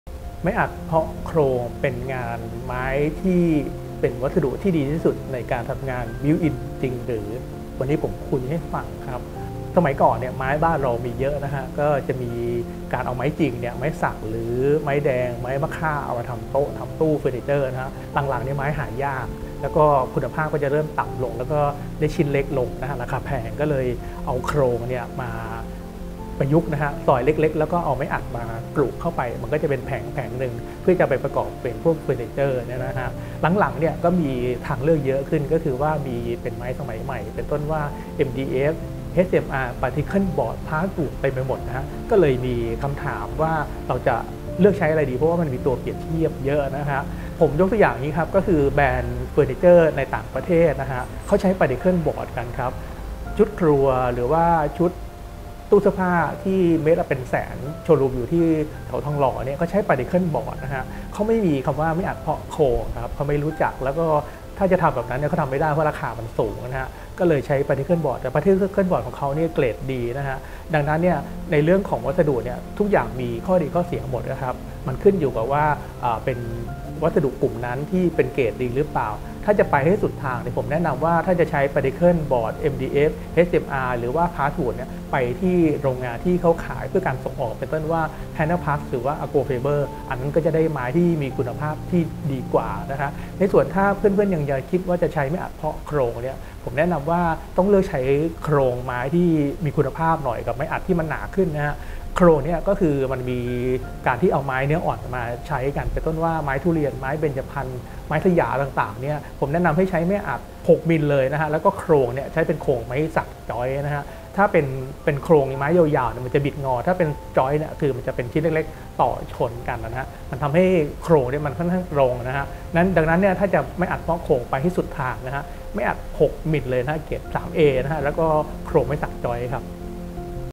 ไม้อัดเพาะโครง ดีที่สุดจริงหรือ?
ในการใช้วัสดุมาทำการตกแต่งภายใน ที่นิยมใช้กันมีไม้อยู่หลายประเภท ไม้อัดเพาะโครงเป็นวัสดุที่เชื่อกันว่าดีที่สุดนั้น จริงหรือ?